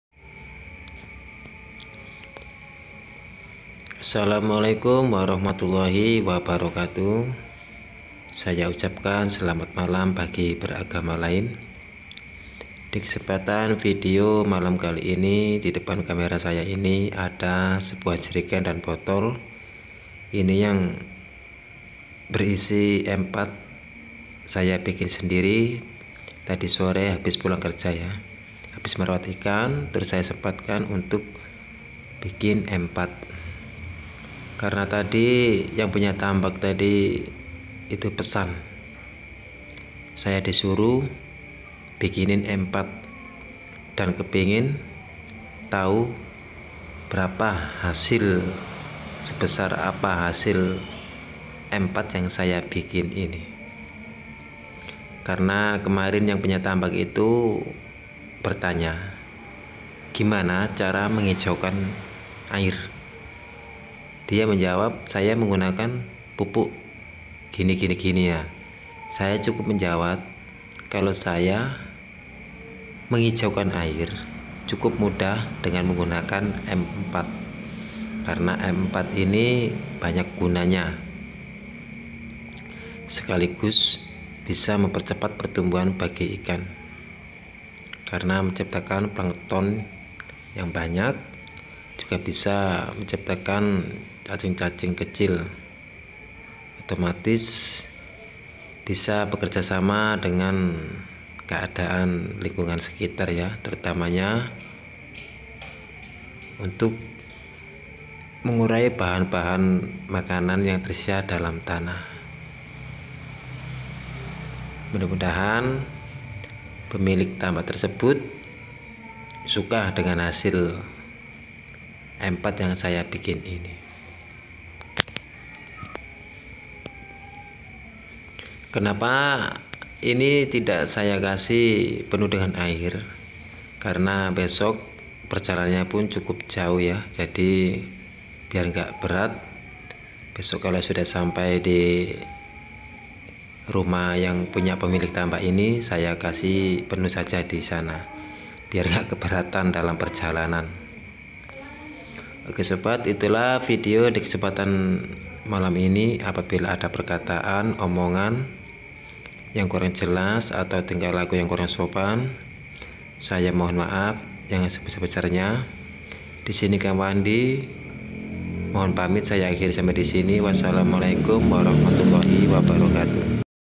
Dapat pesanan probiotik ikan // kanggo wong tambak
di sini kang Wandi dapat tantangan dari orang tambak untuk menghijaukan air, bagi saya cukup mudah tapi di balik warna air yg hijau kita harus tau selain hijau ada manfaat GK bagi ikan, kalau untuk probiotik ikan sudah jelas kandungan" dan manfaatnya jadi tidak ada yg GK mungkin karena probiotik tersebut ..mulai dari bakteri" yg terdapat dalam ramuan yg saya buat lama" bisa menciptakan bakteri yg baik dan menguntungkan bagi semua jenis ikan.